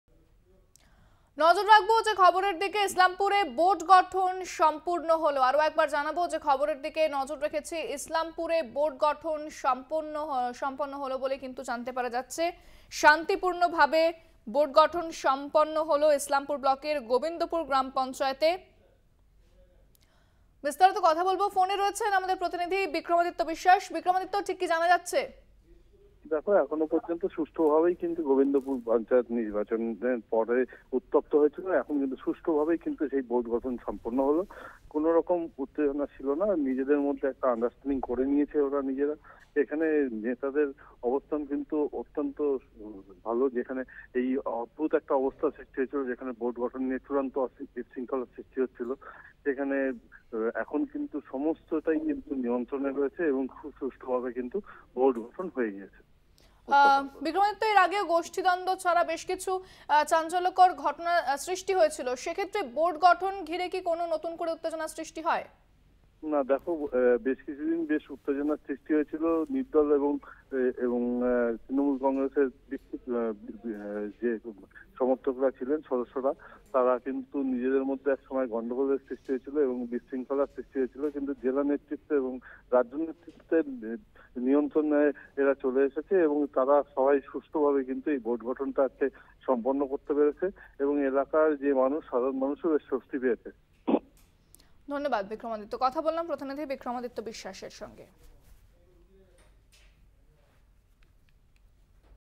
Islampur News | ইসলামপুরে শান্তিপূর্ণ ভাবে সম্পন্ন হয়েছে বোর্ড গঠন প্রক্রিয়া
website link for this copy :
@Kolkata Tv